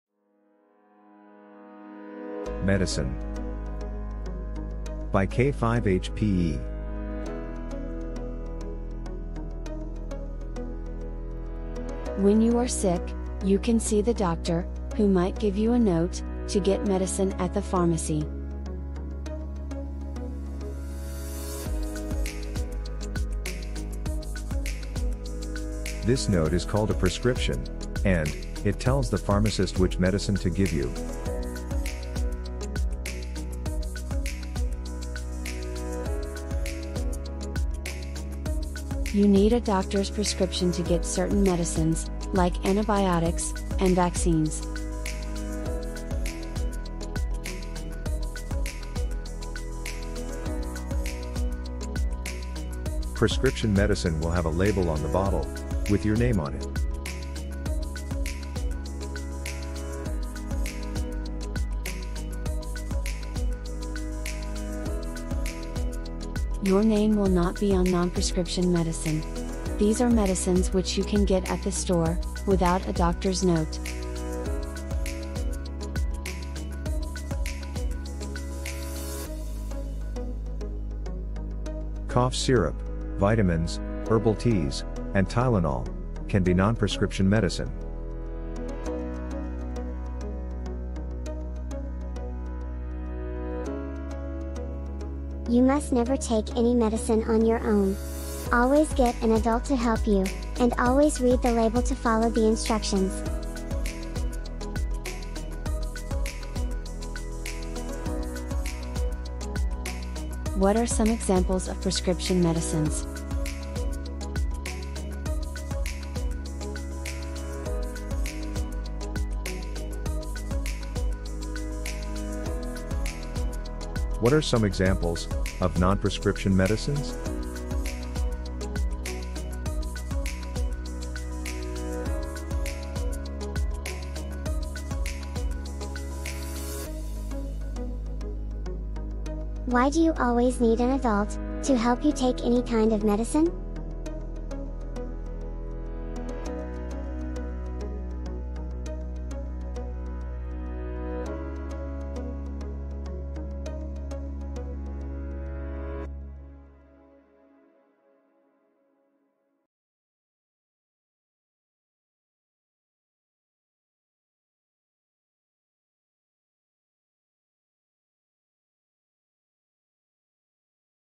Medicine for Kids, Prescription & Non-Prescription, Substance Use and Addictions, Health Education
This video teaches students about Medicine (prescription and non-prescription) with engaging visuals and music. Questions are included at the end to review the substance use and related health concepts.

Ontario Curriculum (Grade Two Health) D1.3 describe the difference between prescription medicines and non-prescription medicines, giving examples of each, and identify rules for the proper use of all medicines